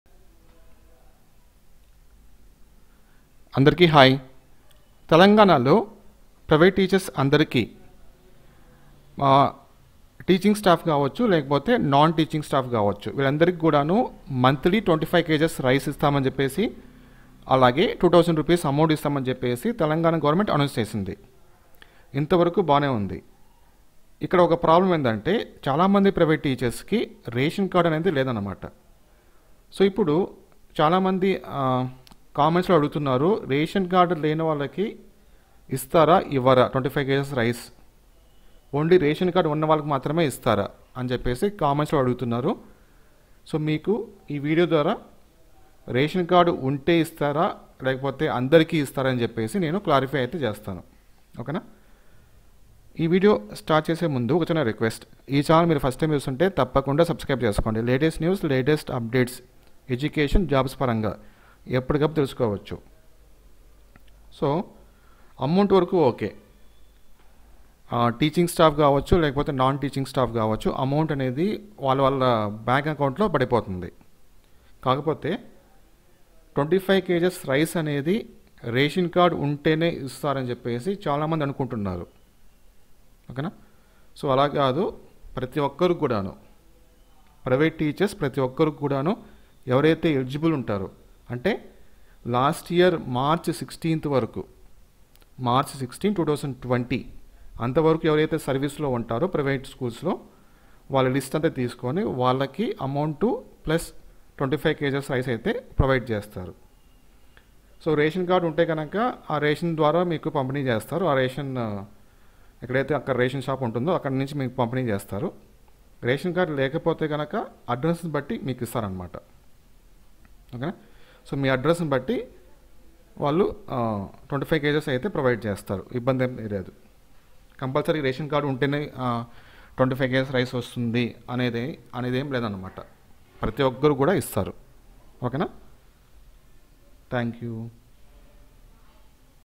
TS Private Teachers 2000 and 25kg rice latest updates || Telangana (TS) private teachers 2000 scheme
HI, TS Private Teachers 2000 and 25kg rice scheme latest updates.